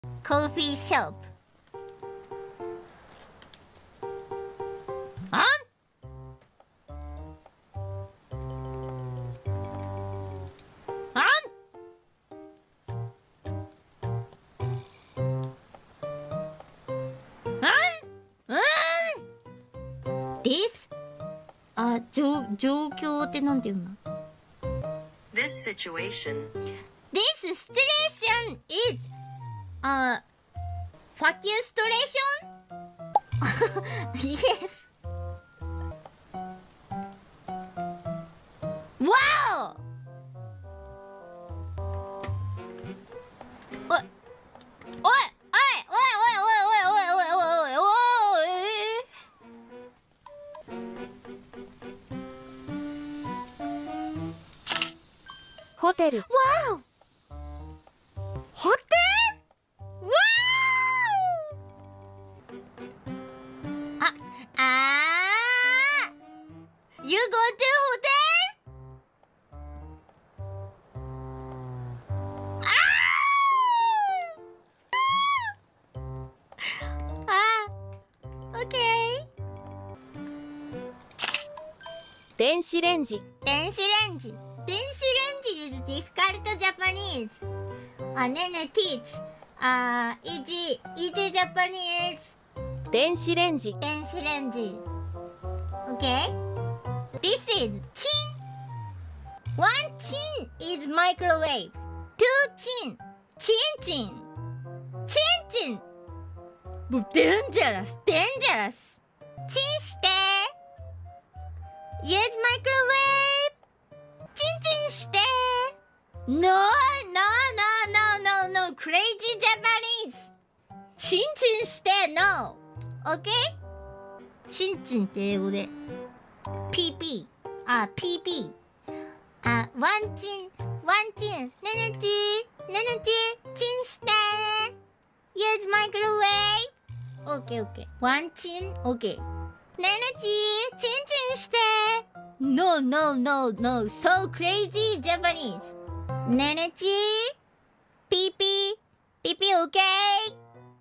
Nene Teaches Her Husbands How To Say "Microwave" But Things Go Lewd Real Quick【Hololive】
▼ Original Stream ▼